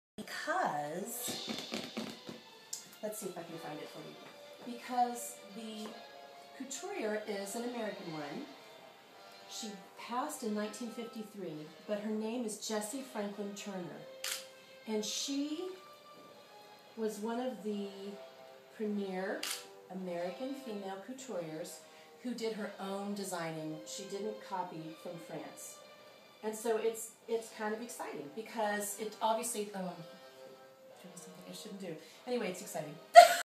Textile Conservator Claudia Walpole discusses early 20th century American couture part 3
Behind the scenes with Claudia Walpole, Textile Conservator, as she prepares for "Star Power: Edward Steichen's Glamour Photography," on view at Reynolda House Museum of American Art, 2/23/13 through 5/19/13.

ReynoldaHouse.org